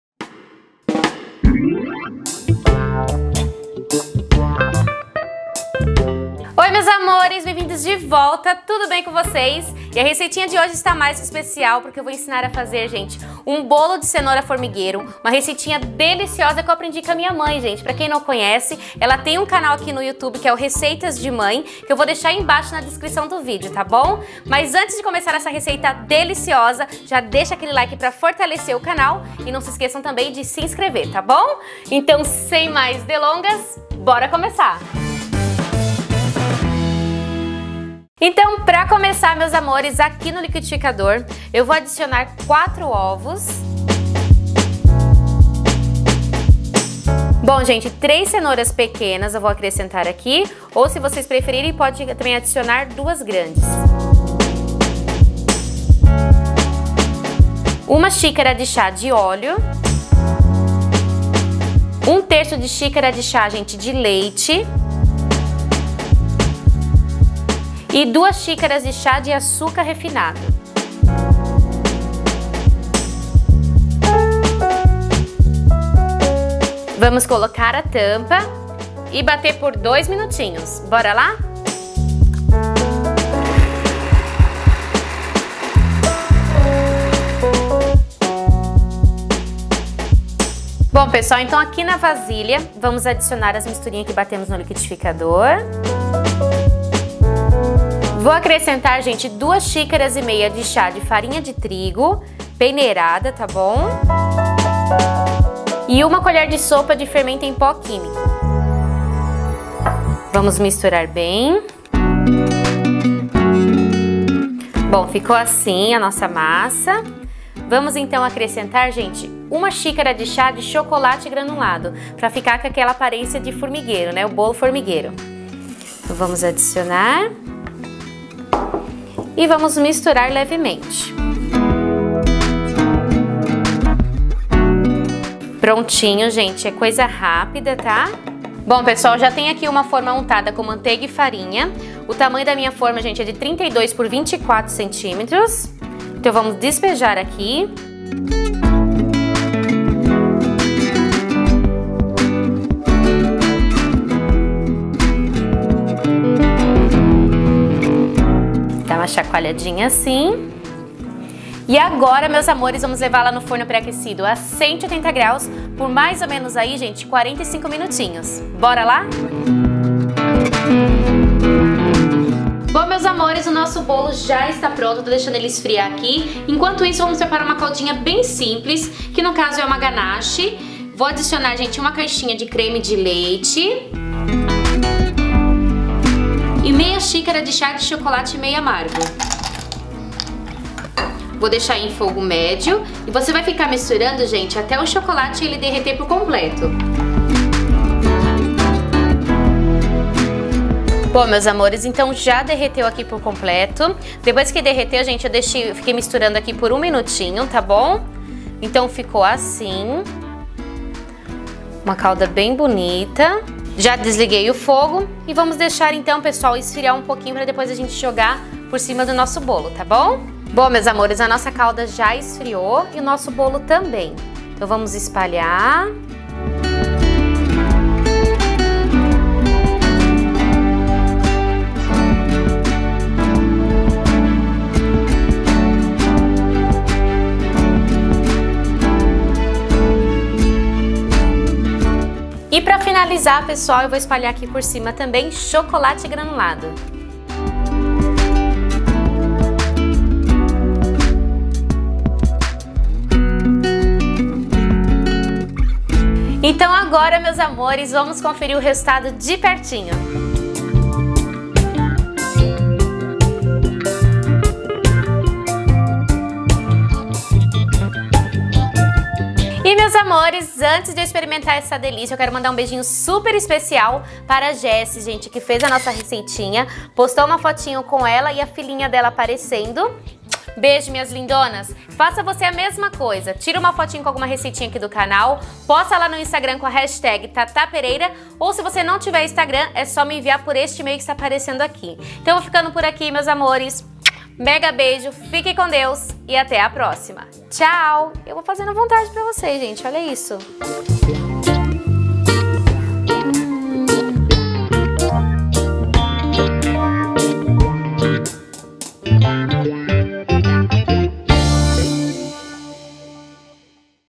BOLO FORMIGUEIRO DE CENOURA SUPER FÁCIL
Oi meus amores! E a receitinha de hoje está mais que especial, pois vou ensinar uma receitinha de família, um delicioso BOLO FORMIGUEIRO DE CENOURA, super fácil e perfeito para tomar com aquele cafezinho : ).

Carrot anthill cake, super easy

Para envios de correspondências e entregas:
Tata Pereira
Caixa Postal:1707
Sorocaba SP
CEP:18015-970

❤Os ingredientes são:
PARA MASSA:
4 ovos
1/3 xícara (chá) de leite
3 cenouras pequenas cortada em rodelas(400g)
1 xícara (chá) de óleo
2 xícaras (chá) açúcar
2 xícaras 1/2 (chá) de farinha de trigo peneirada
1 colher (sopa) de fermento em pó químico
1 xícara (chá) de chocolate granulado
Forma untada com manteiga e farinha
COBERTURA:
1 caixinha de creme de leite
1/2 xícara (chá) de chocolate meio amargo ou ao leite
Chocolate granulado
Tamanho da forma: 32 x 24 cm